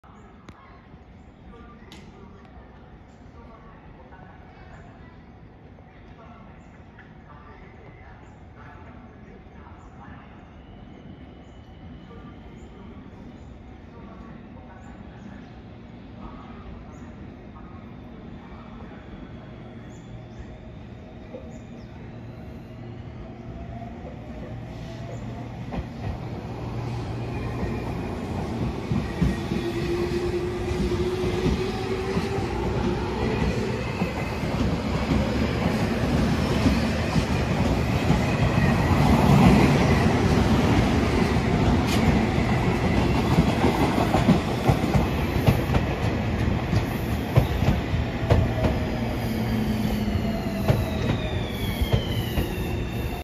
【下板橋電留線からの出庫】下板橋の留置線から池袋駅へ送り込まれる回送列車
撮影日：2024年9月24日
撮影場所：下板橋駅
車両：東武30000系
編成：31615編成+31415編成 (31615F+31415F)